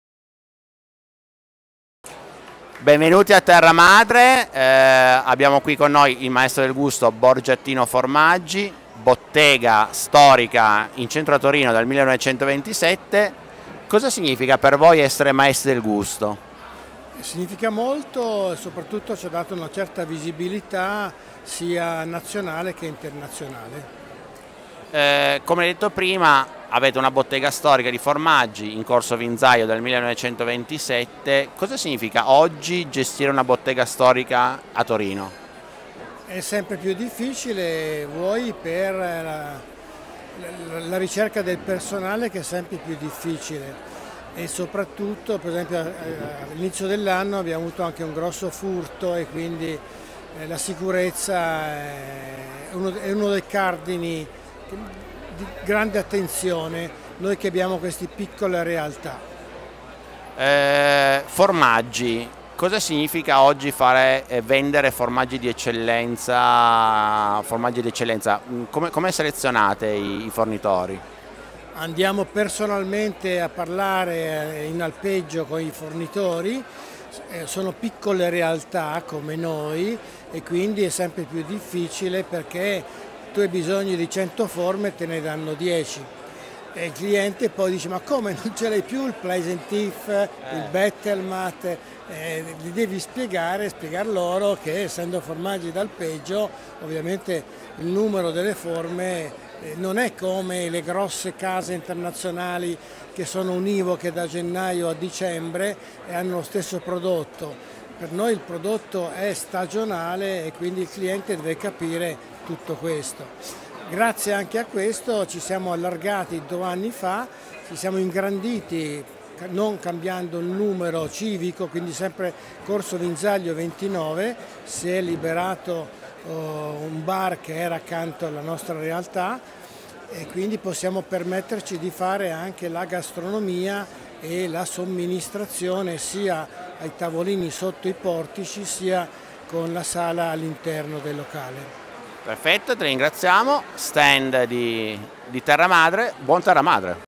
Luciano Guidotti (Borgiattino Formaggi) – Terra Madre Salone del Gusto 2024
Luciano Guidotti intervistato allo stand camcomtorino durante Terra Madre Salone del Gusto 2024